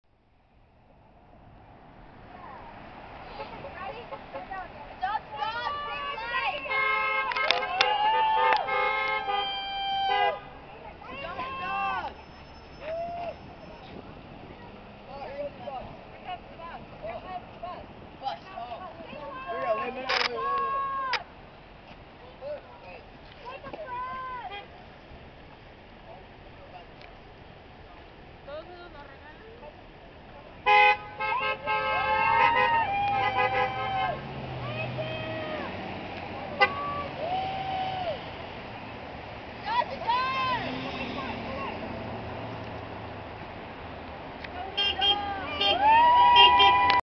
Video2 Kids Against Animal Cruelty with Animal Advocates Alliance Adoptions 8-1-10
Kids trying to bring attention to animals from the Baldwin Park shelter that need adopted so they are not euthanized.